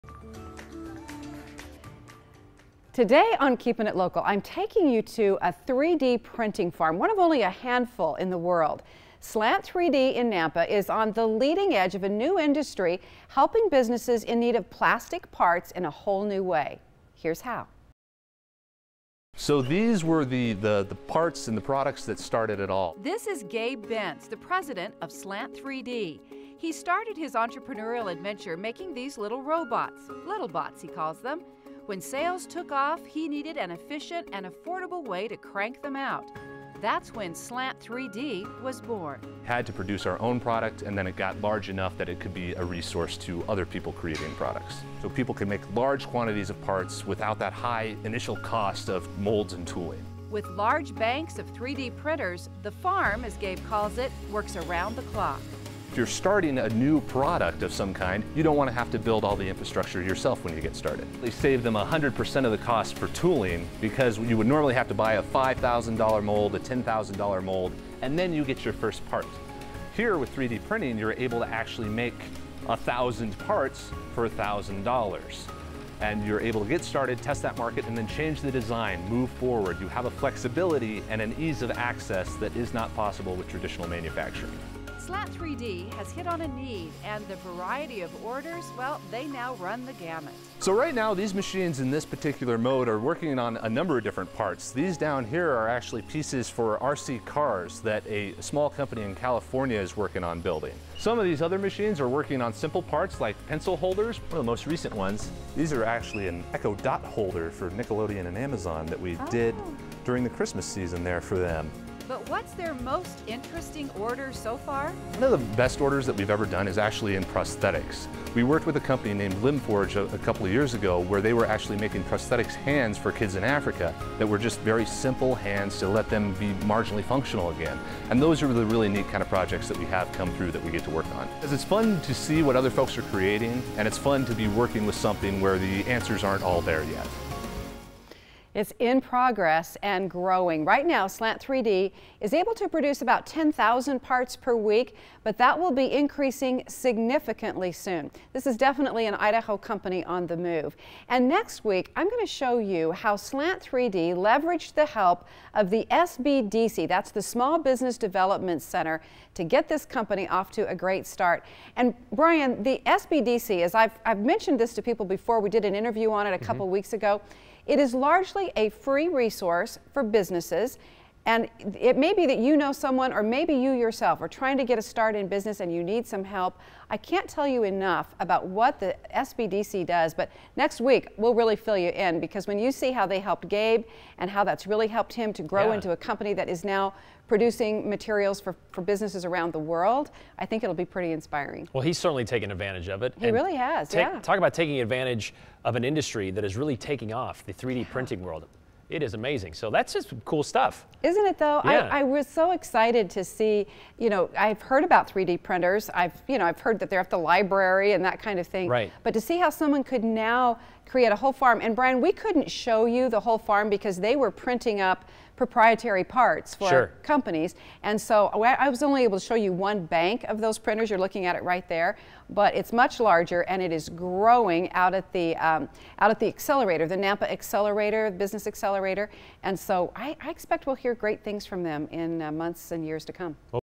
Keepin' It Local: 3D printing farm in Nampa
Slant 3D is an Idaho company on the move.